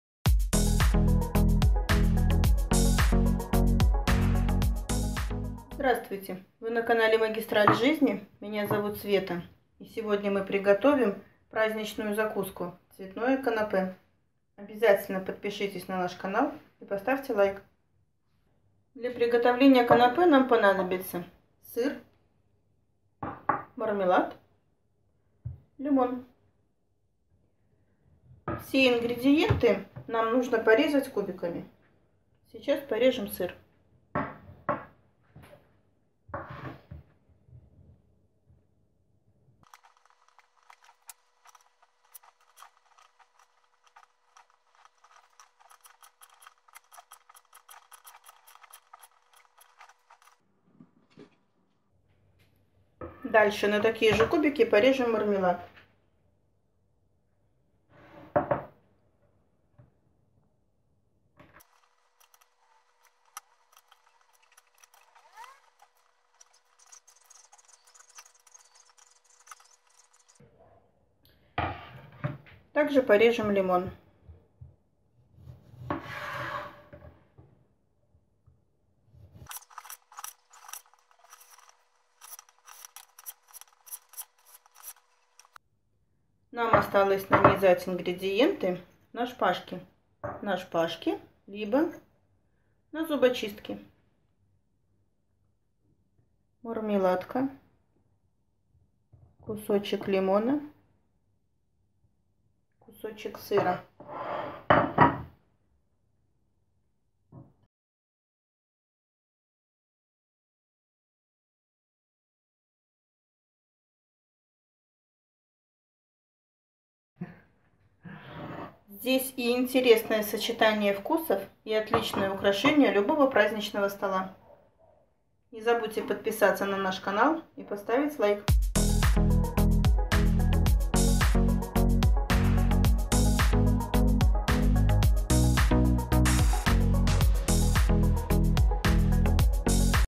ЗАКУСКА сырная. Всего 3 ИНГРЕДИЕНТА. Простой, вкусный рецепт канапе с сыром на праздничный стол.
Россия:
💳 Карта МИР СберБанк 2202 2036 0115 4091

Беларусь:
💳 Карта VISA Альфа-Банк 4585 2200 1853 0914

В этом видео пошаговый очень простой, быстрый и вкусный  рецепт закуска к  праздничному столу с сыром.
Поэтапно показано как приготовить этот вкусный, простой и быстрый рецепт из сыра.
Пошаговый рецепт закуски канапе и сыр быстро, легко и просто приготовить нужны всего лишь...

Ингредиенты
Мармелад
Сыр
Лимон